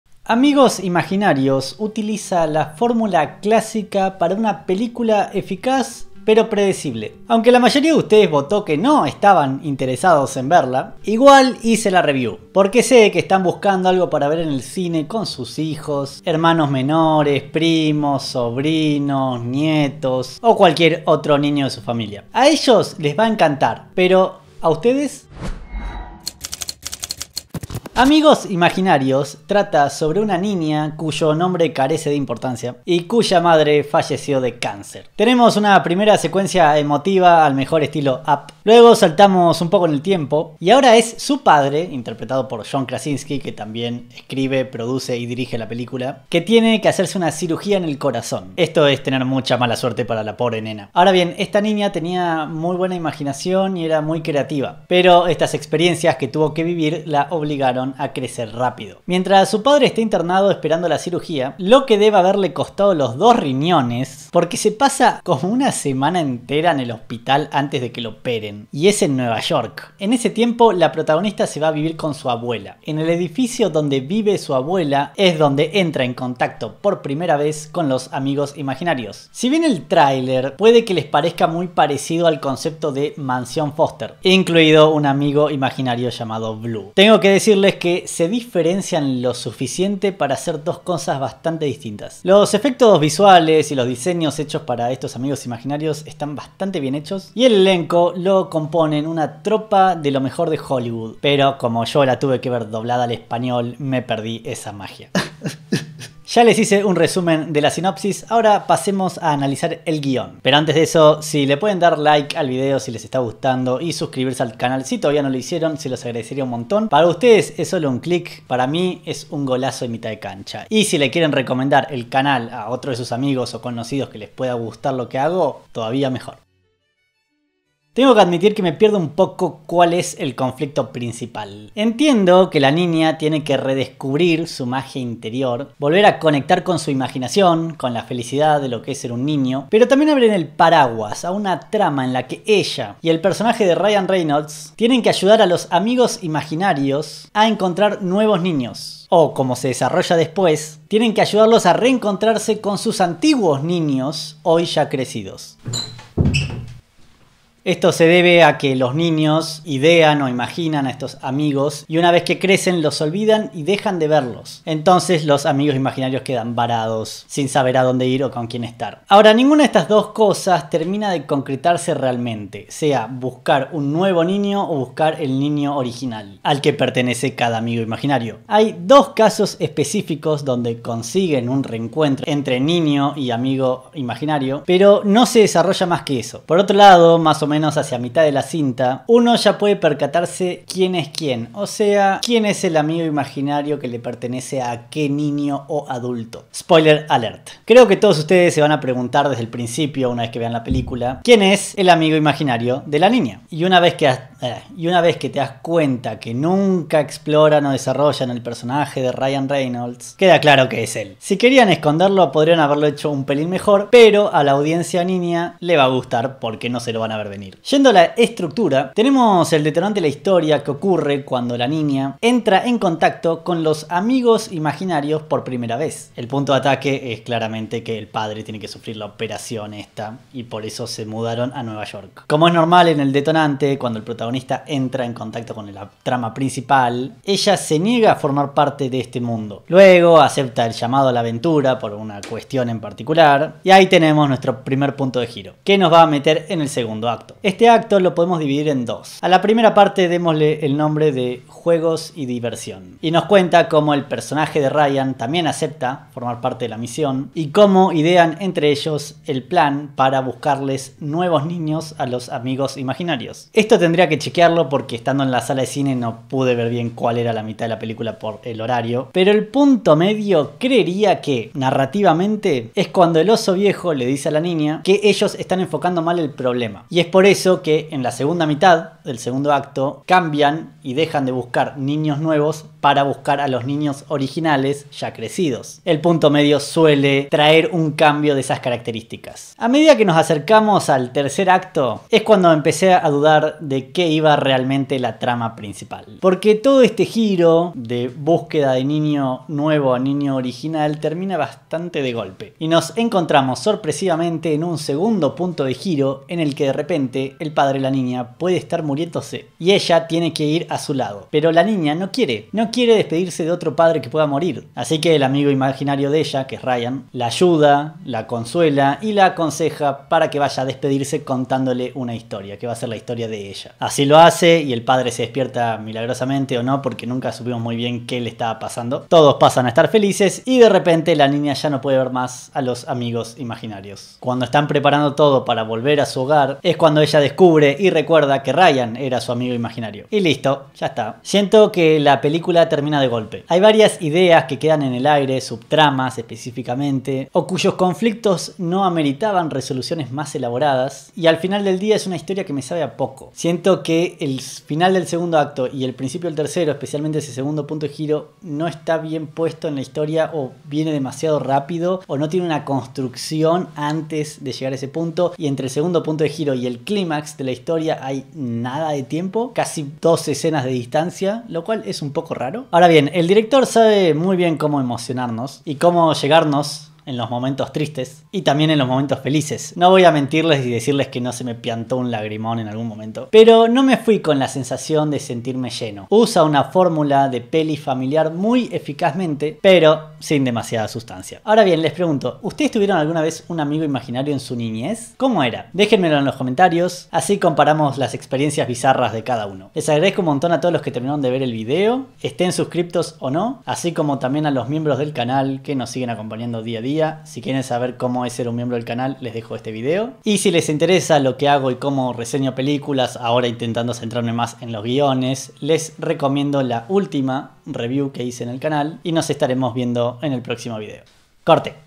🟧 AMIGOS IMAGINARIOS usa la fórmula perfecta para ver en familia
John Krasinski vuelve a dirigir y actuar, esta vez con una película para toda la familia, que cuenta la historia de una niña que puede ver a los AMIGOS IMAGINARIOS que se fueron perdiendo una vez sus niños empezaron a crecer.

¿Ya la vieron? ¿Qué les pareció? ¡Déjenlo en los comentarios!